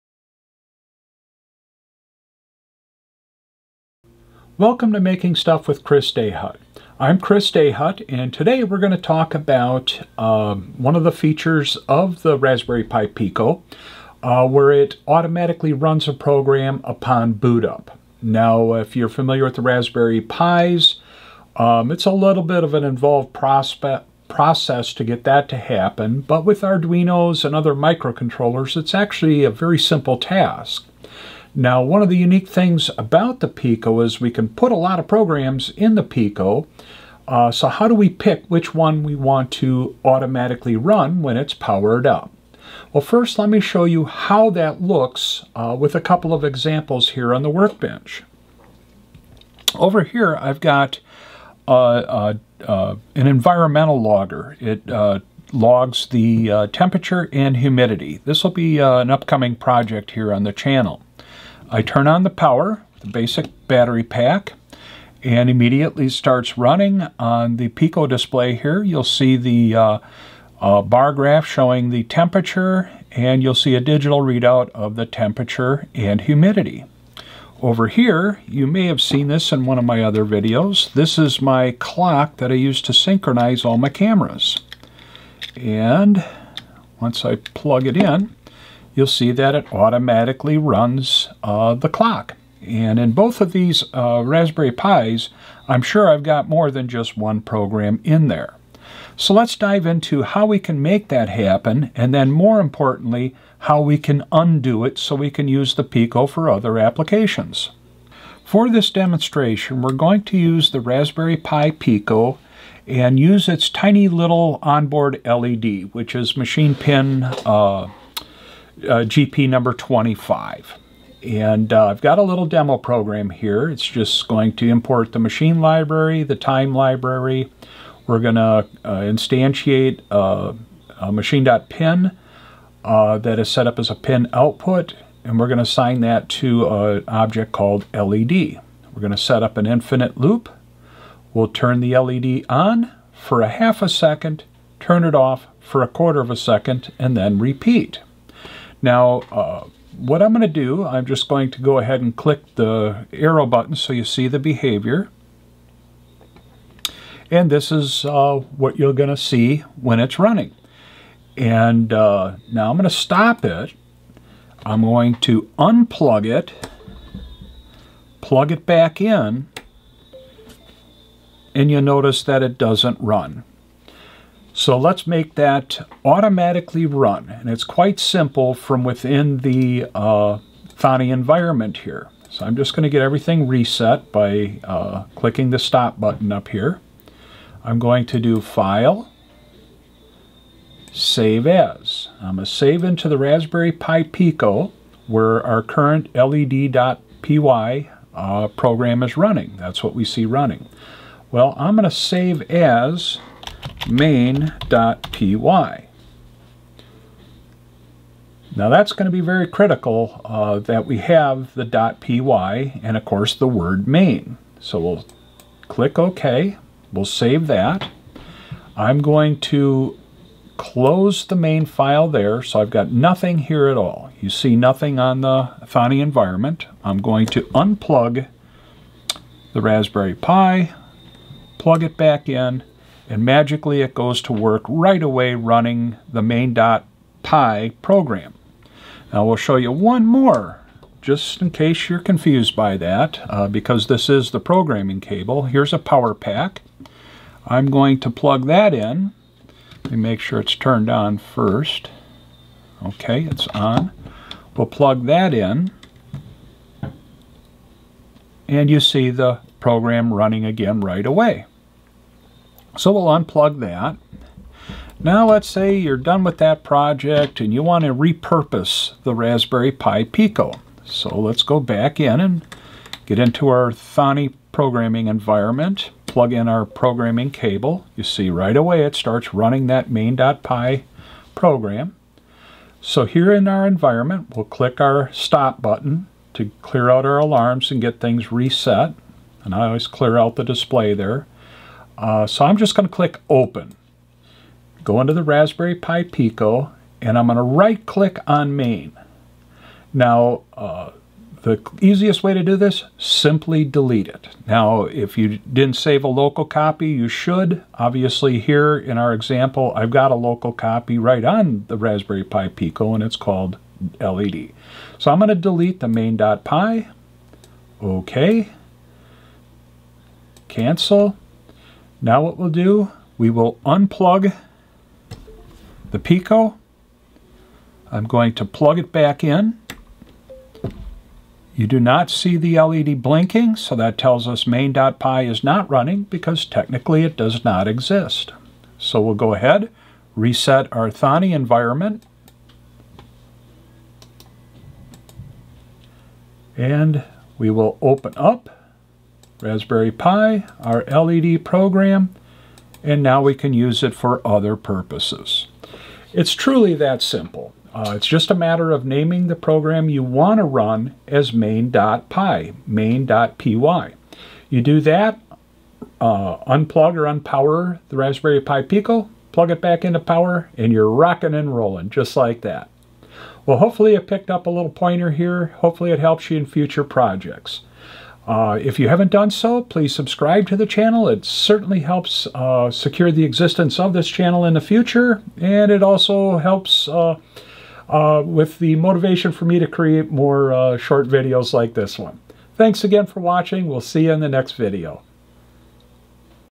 How to make the RPi PICO autorun a program
In this video, Chris demonstrates how to make a program auto run on power up.  Also covered is how to stop the program from auto starting as well.